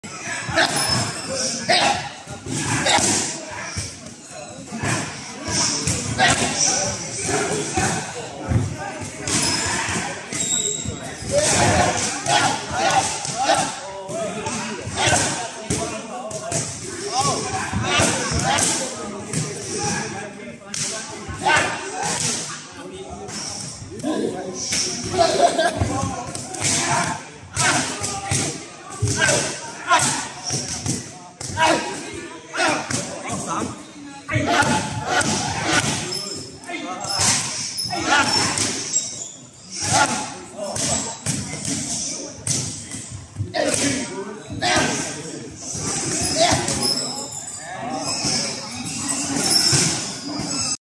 Muay Femur pad work from Sajad Sattarii
Muay Femur pad work from Sajad Sattari and coach Chris Forster at Venum Training Camp Thailand.